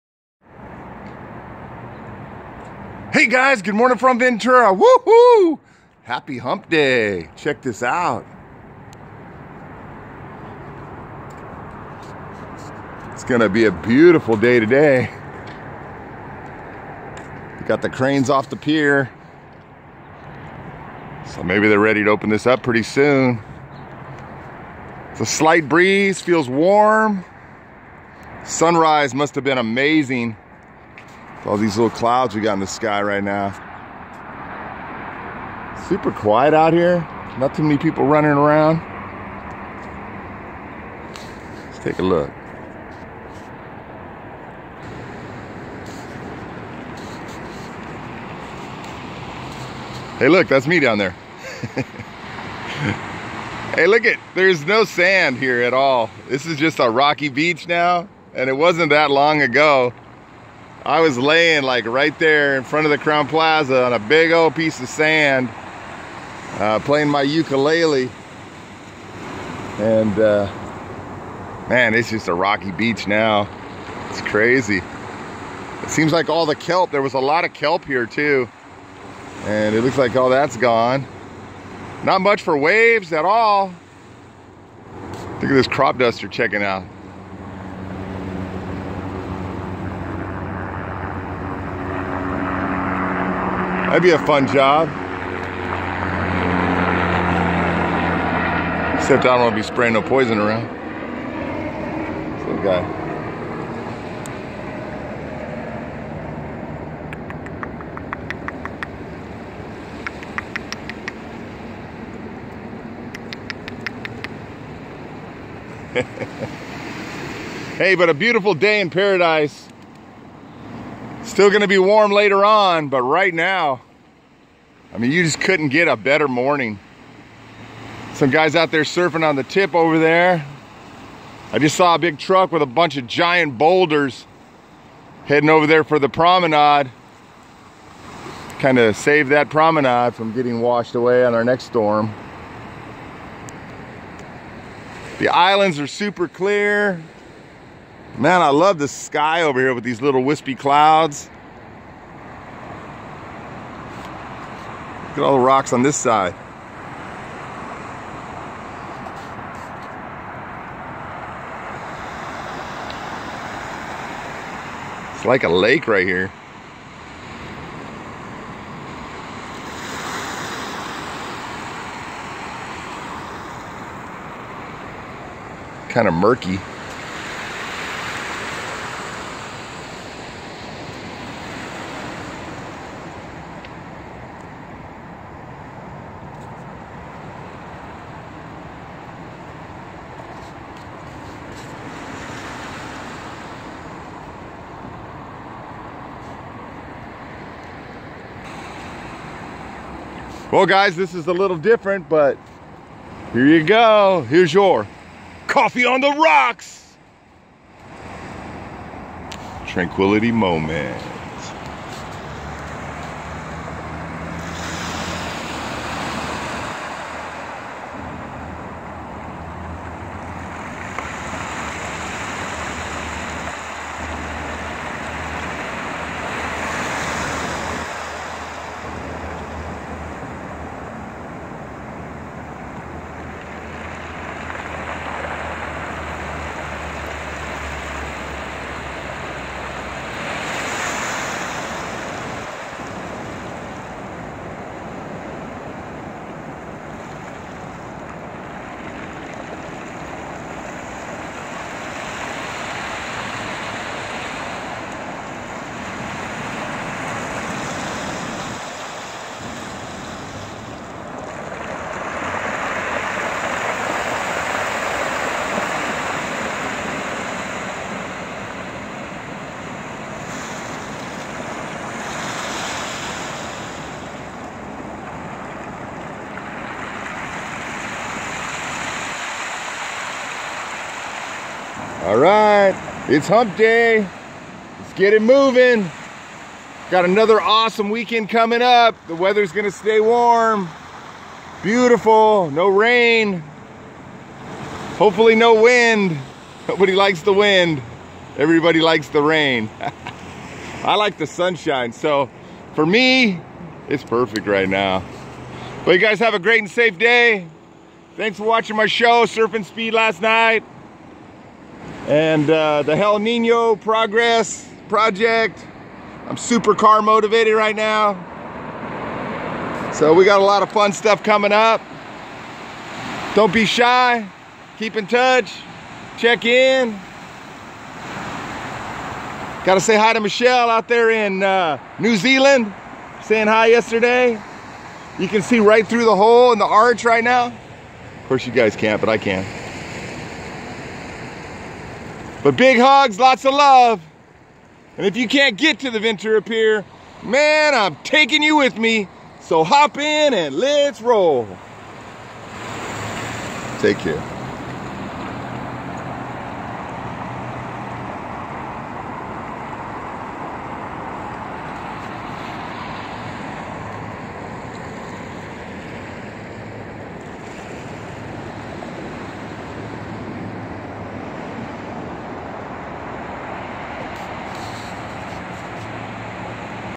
Happy Hump Day! Live from the Ventura Pier!
The heat wave continues as come along as we walk out on the pier and take a close look at the Channel Islands and Ventura Beach...